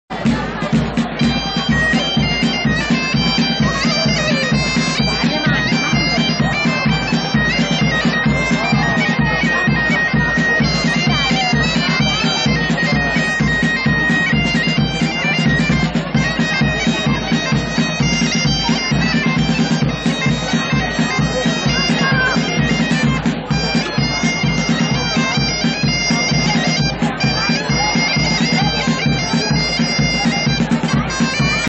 LY SIETECOLORES. La dansa del foc. Saltimbanquis de Sambi
Mercat Medieval de Capdepera 2014